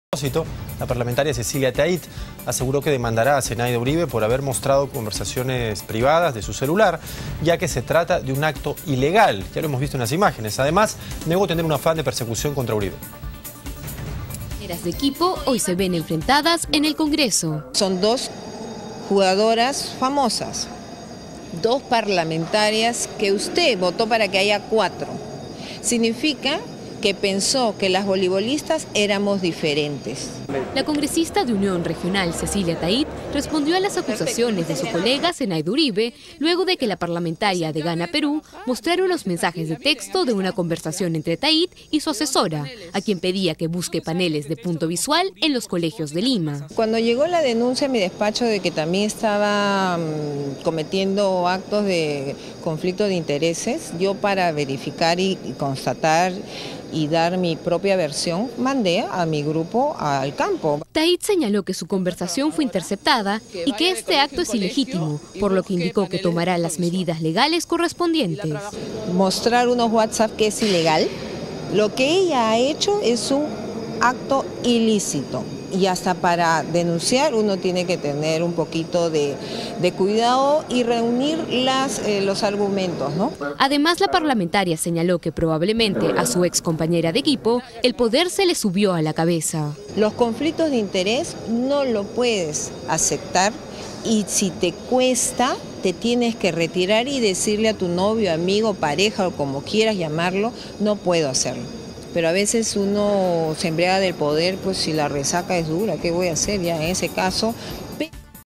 Cecilia Tait demandará a Cenaida Uribe por mostrar mensajes privados
La congresista de Unión Regional respondió a las acusaciones en su contra sobre un supuesto complot.